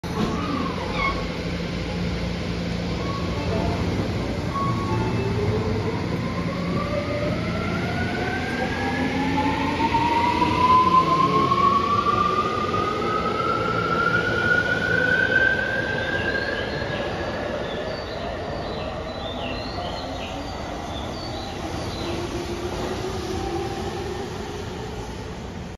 東京メトロ１７０００系和光市駅発車シーン
２０２１年１１月１４日撮影。東京メトロ１７０００系の和光市駅発車シーンです。１７０００系は２０２１年デビューで老朽化した７０００系を取り替える為に製造されました。